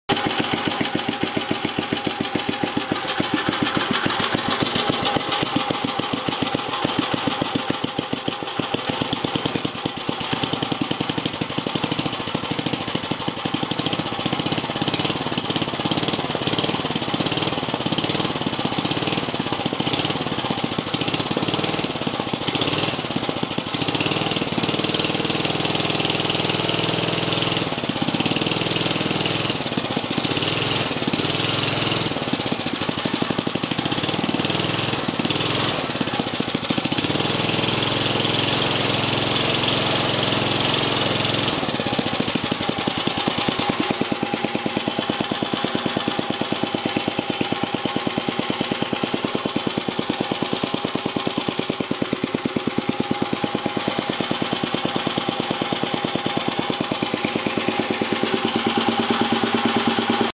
Go Kart project, engine test fit.
Not long after I got this project go kart. I wanted to test fit the motor before completely rebuilding this go kart and the engine. 5HP Briggs & Stratton flathead motor from a snow-blower. Date code on the engine indicates original build year of 1979.